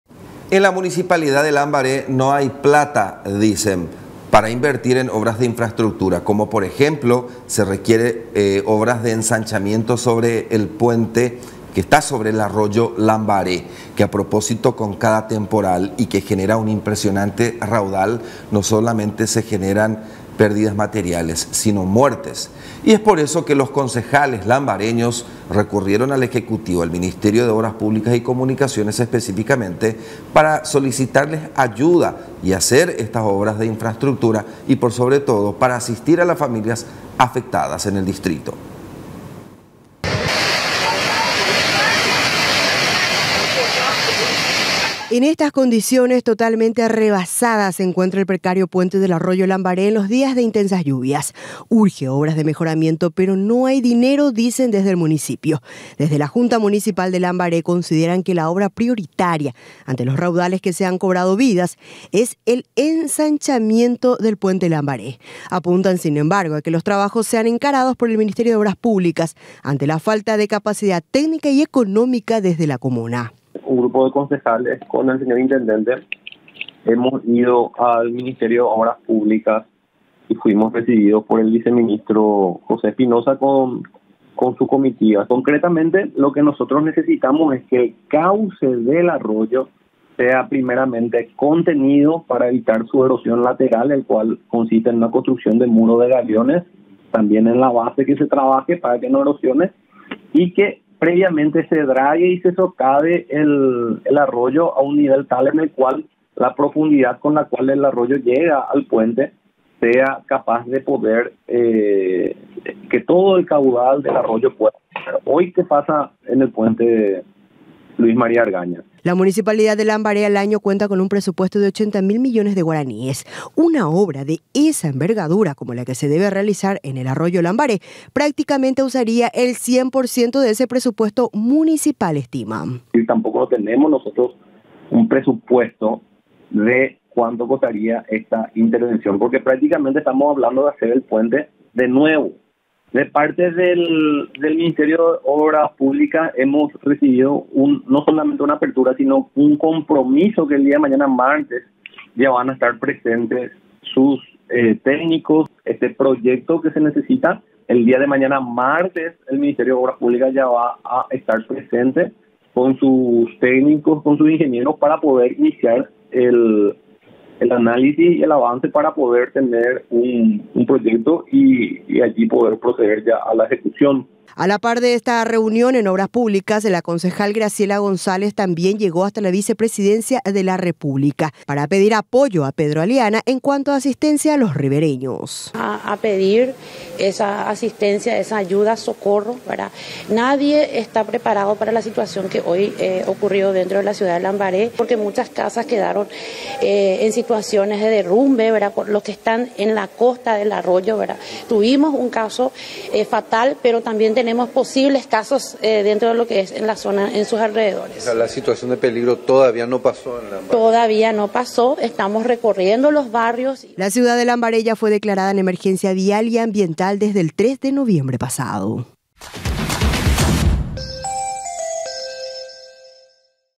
Lambaré: municipio está sin dinero para obras en arroyo Lambaré
Hay obras urgentes que se deben encarar en Lambaré, tras el temporal, entre ellas se encuentra el ensanchamiento del puente sobre el arroyo Lambaré, pero no hay dinero municipal. Concejales recurren al Ejecutivo para obras y asistencia a los damnificados de las zonas ribereñas de este distrito.